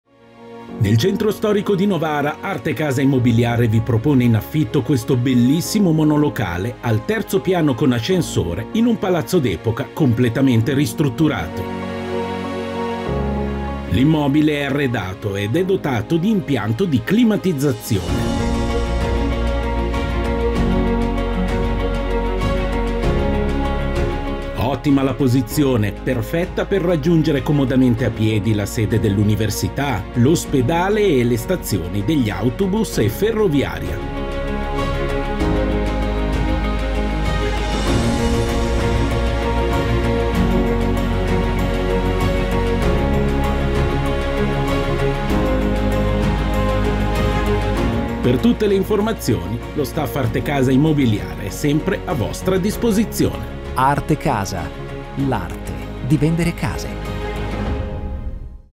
NOVARA, CENTRO STORICO - In affitto monolocale arredato
In affitto a Novara in centro storico monolocale arredato in palazzo d'epoca ristrutturato.

I nostri contatti:
🏠 Novara, via G. Morera 5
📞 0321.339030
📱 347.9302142
🌍 social: artekasaimmobiliare
🖥 skype: segreteria.artekasa